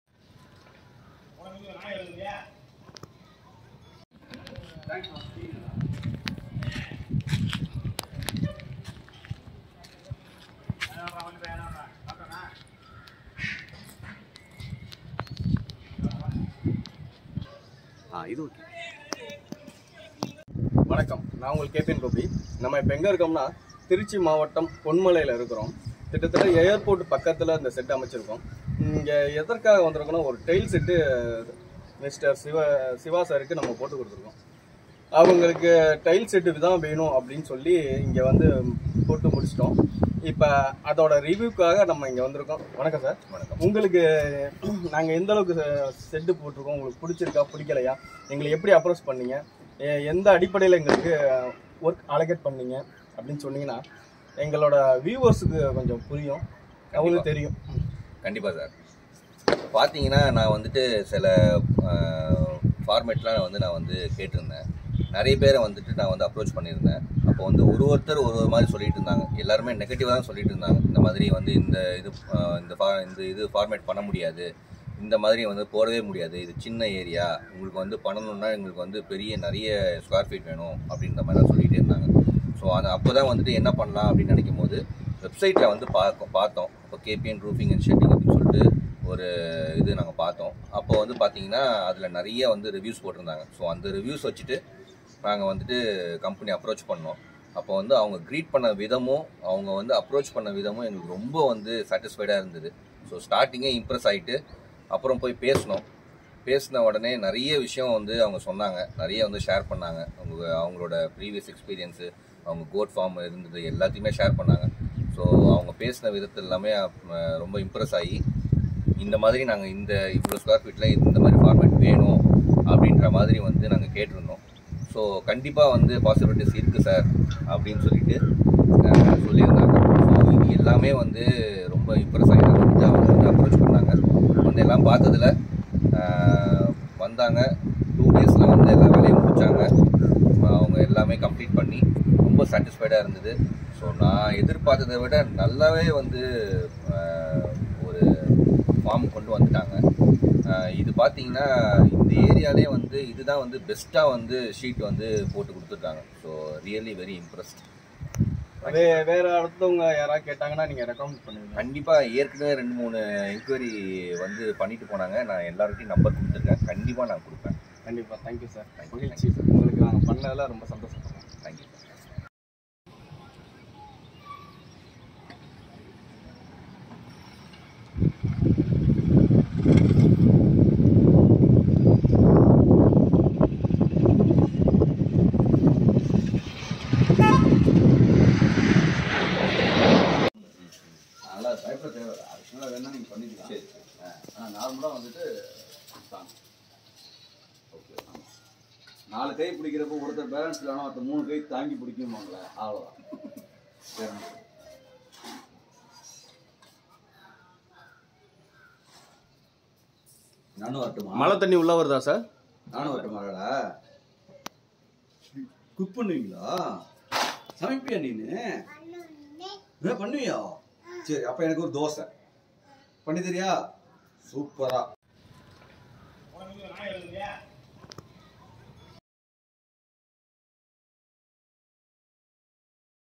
Low Budget Kerala Shed | Tile Shed | Terrace Shed
Tile Model Shed Trichy/ Tamilnadu

Size: 15'x18'

Customer Name: Mr.Siva Shankar

Company: KpN Roofing Shed

         Plan: Mr.Gopi

  Engineer: Mr.Raja Ram

Production: Mr.Yogaraj

Contractor: Mr.Arun

    Drawing:Mr.Ganesh

Customer review:Good 5⭐️

More Details :-
KpN Roofing Shed